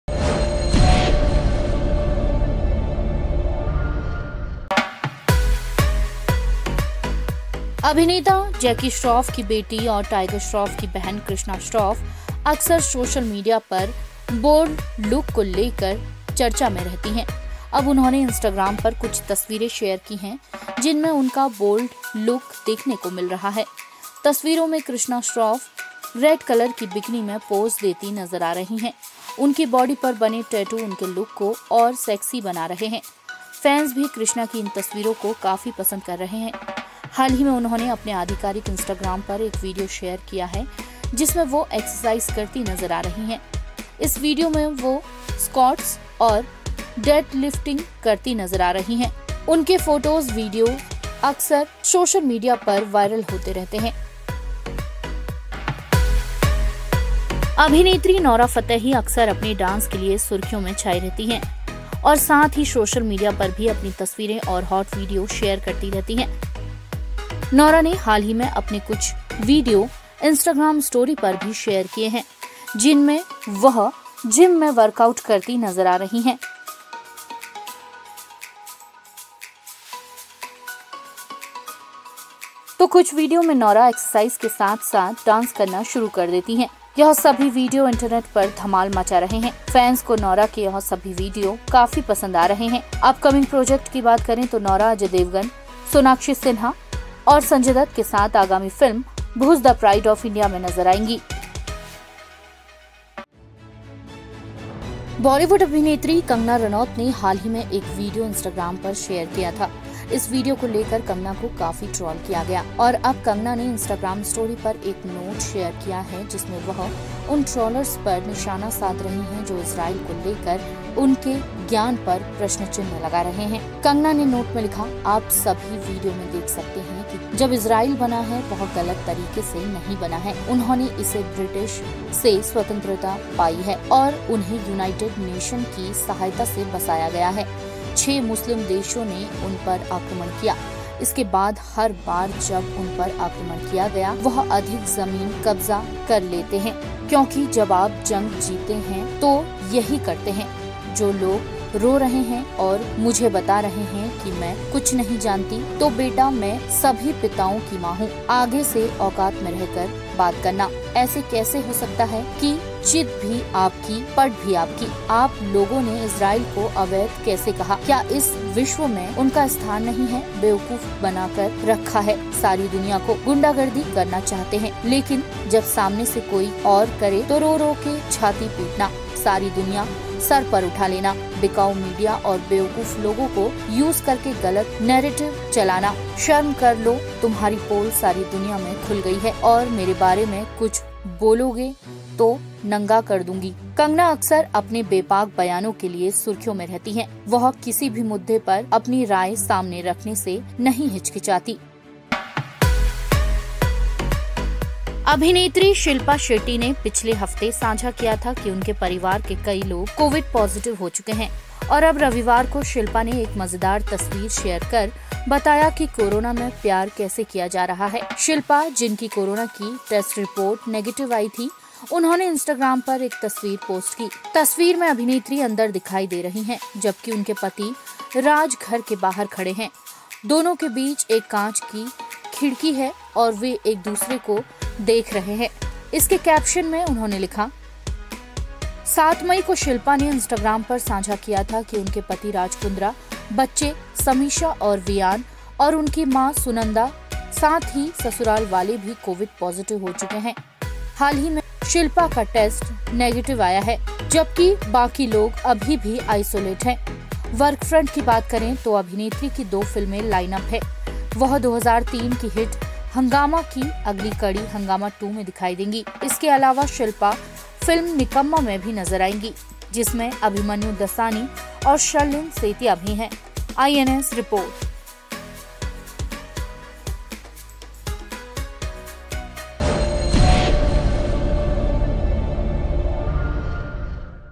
Krishna Shroff ने सोशल मीडिया पर शेयर की Bold Pic | Nora ने Gym में किया Belly Dance वीडियो viral
अभिनेता जैकी श्रॉफ की बेटी और टाइगर श्रॉफ की बहन कृष्णा श्रॉफ सोशल मीडिया पर बोल्ड लुक को  लेकर चर्चा में रहती हैं।